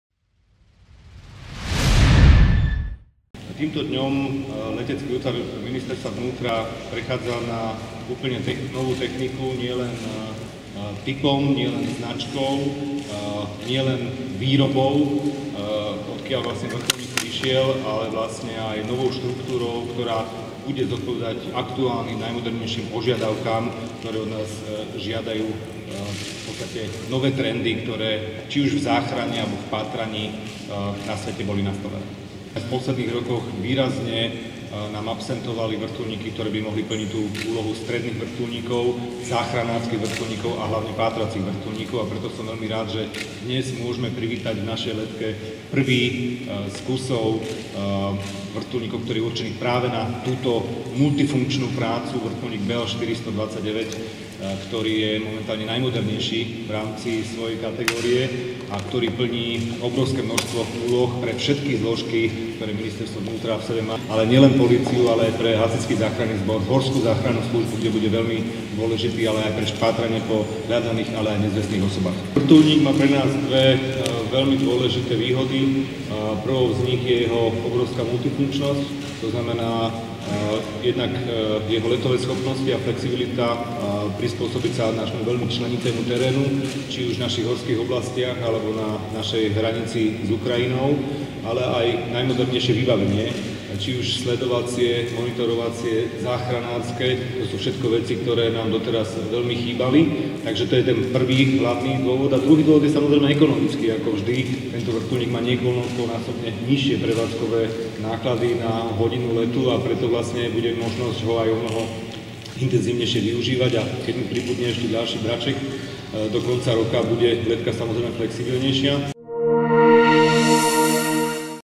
Tento nový stroj bude hľadať nezvestných i migrantov
Policajti, záchranári i hasiči sa môžu tešiť na nový stroj. Kľúče od helikoptéry Bell 429 si dnes slávnostne prevzal minister vnútra Robert Kaliňák (Smer-SD). Vrtuľník za viac ako päť miliónov eur bude monitorovať cestných pirátov i migrantov na hraniciach s Ukrajinou.